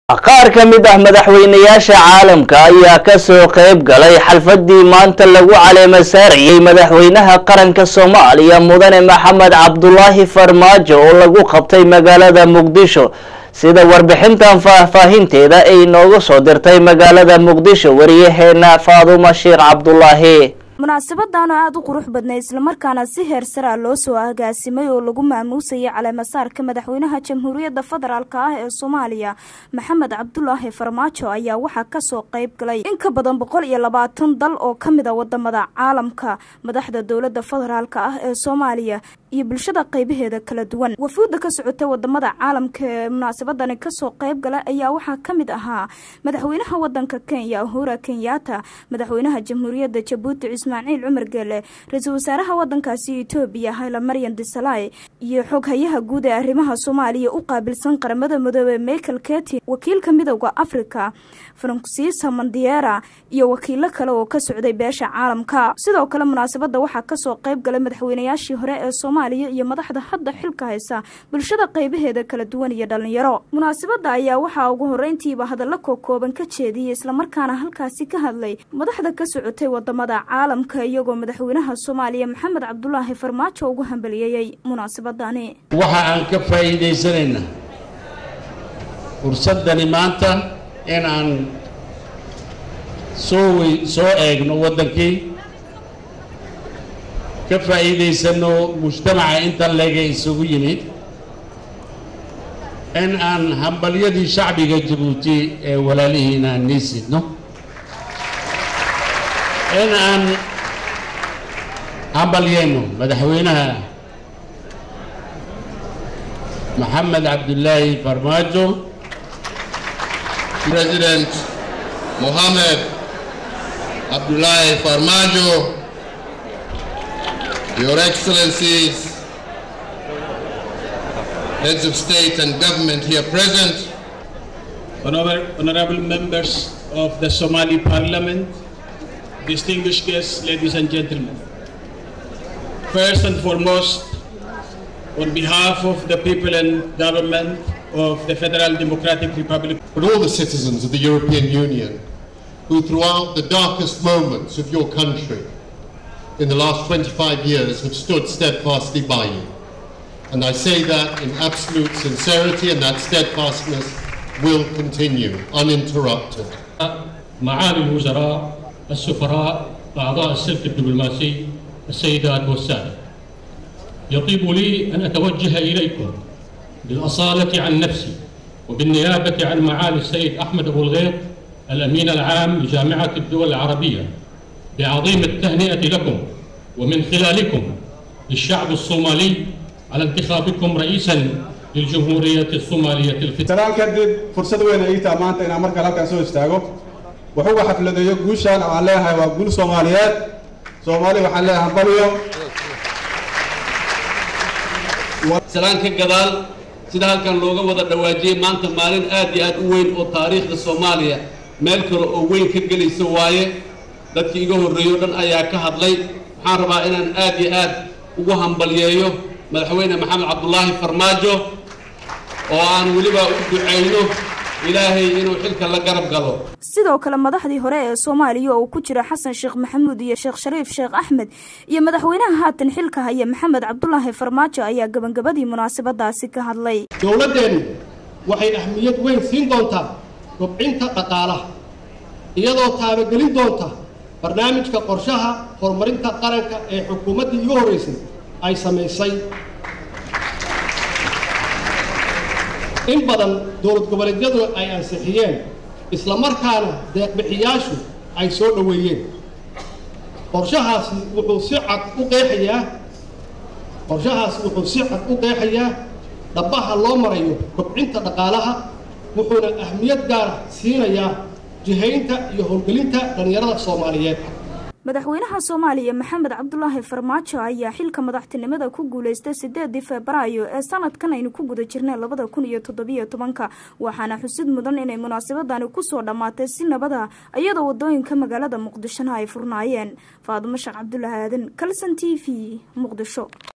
Xafladdii caleemo saarka madaxweyne farmaajo sidey u dhcadya waarbixin
Xafladdii caleemo saarka madaxweynaha qaranka Soomaaliyeed mudane Mohamed Abdullahi Farmaajo sidey u dhcadya waarbixin